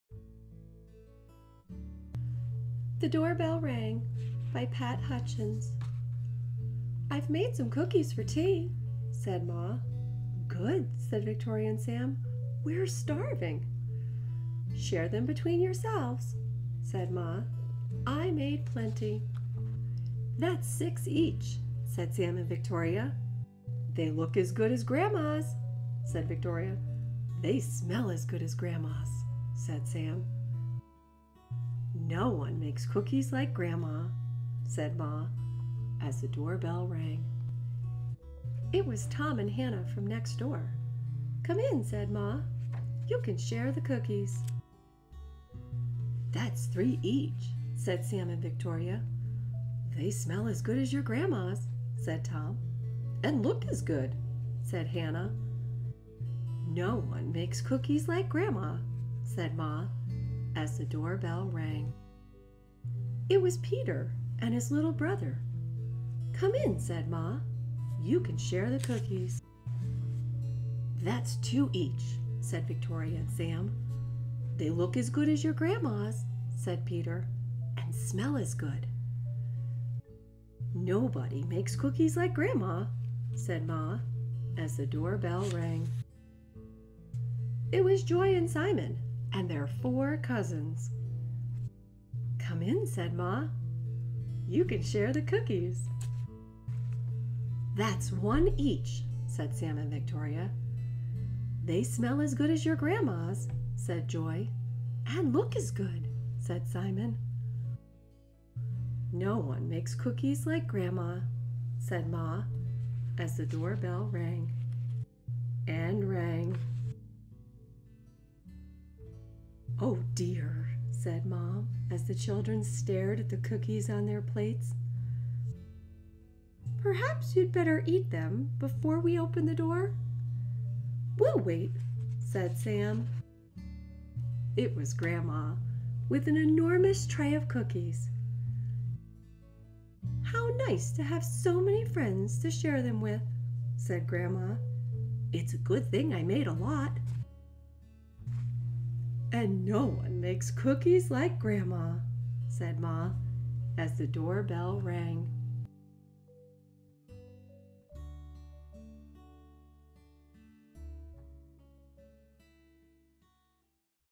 The Doorbell Rang | Pat Hutchins | Children's Read Aloud | Math Story
The Doorbell Ran by Pat Hutchins. Ma make cookies for tea and Victoria and Sam were going to share them. Before they could have the cookies the doorbell rang. They invited them to share in the cookies. Again before they could have the tea and cookies the doorbell rang and more children came to share in the cookies.